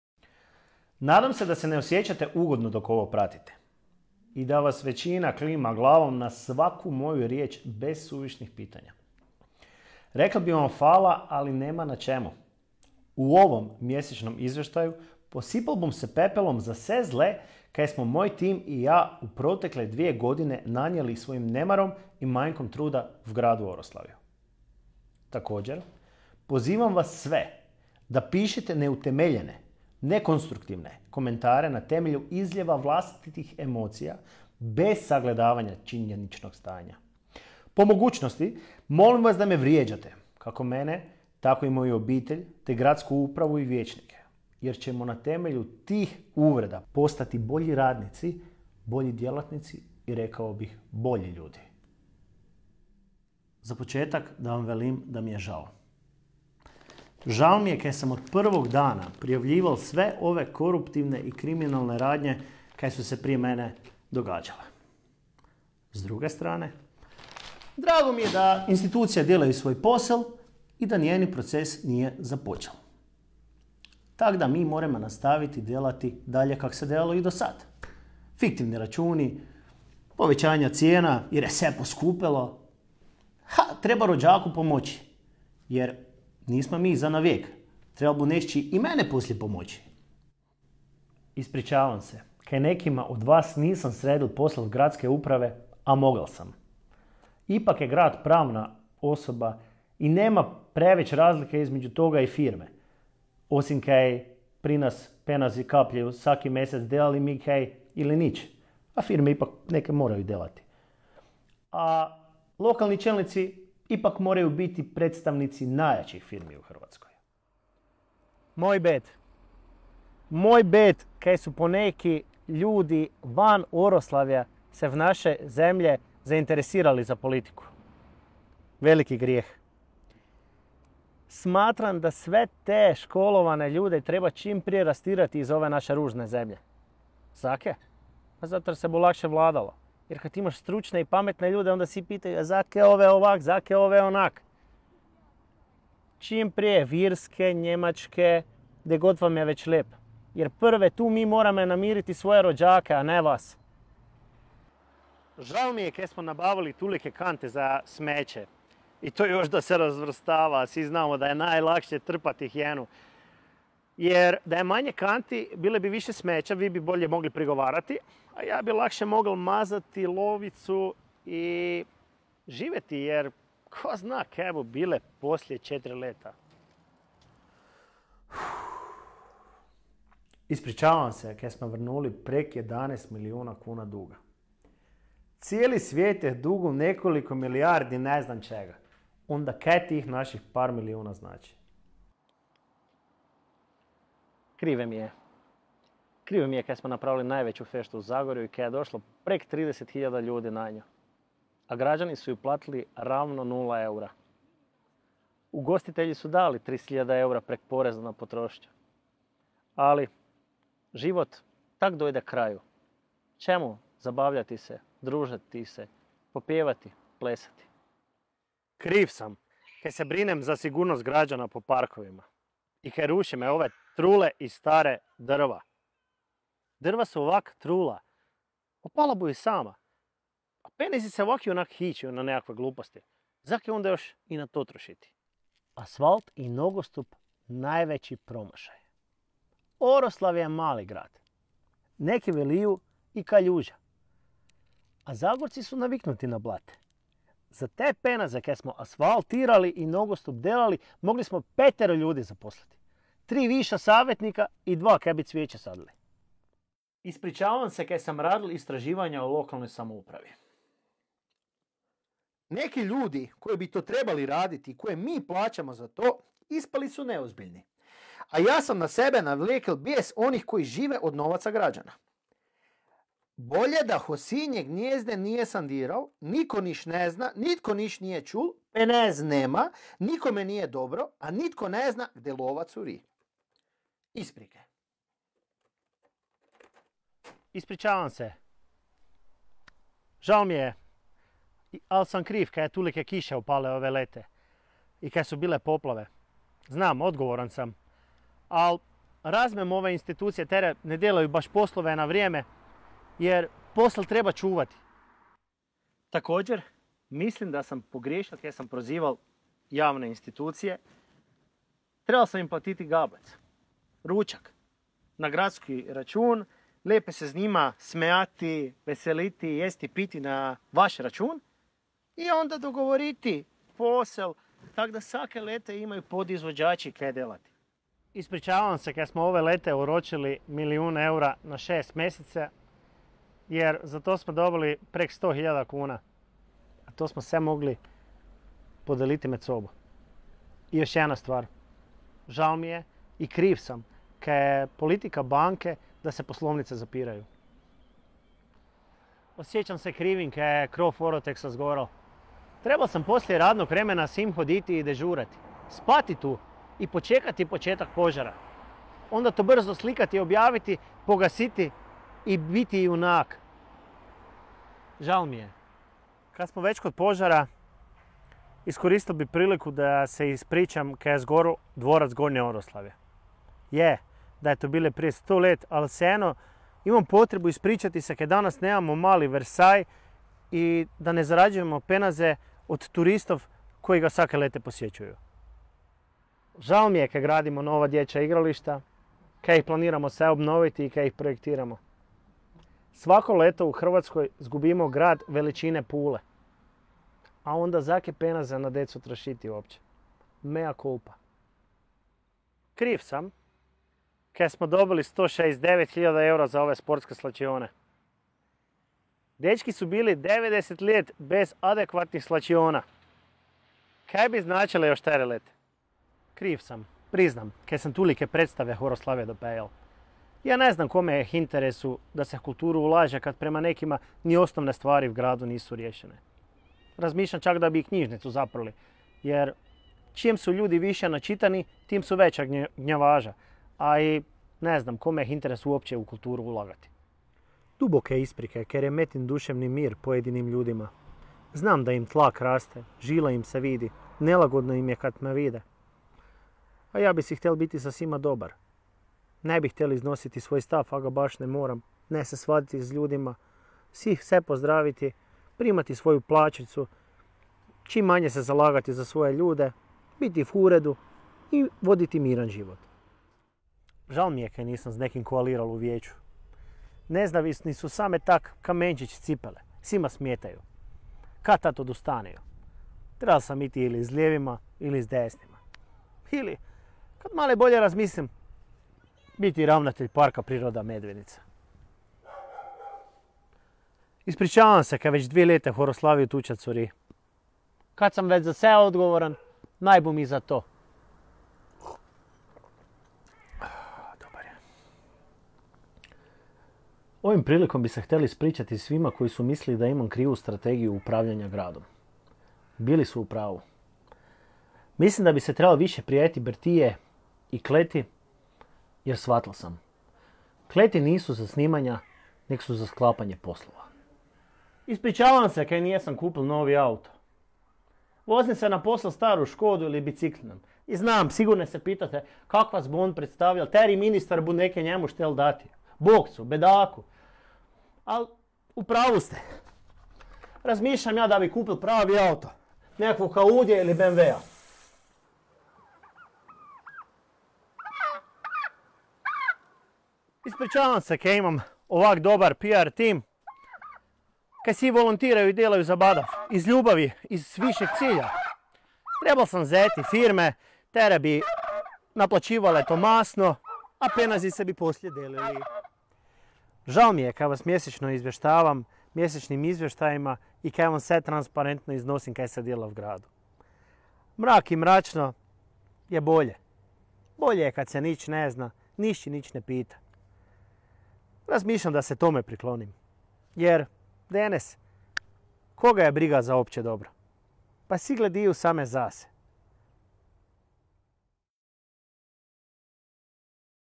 Viktor Šimunić se posuo pepelom
Gradonačelnik Oroslavja Viktor Šimunić u ovomjesečnom izvješću sprdao se je sa svojim kritičarima... Više na
(Video: Viktor Šimunić)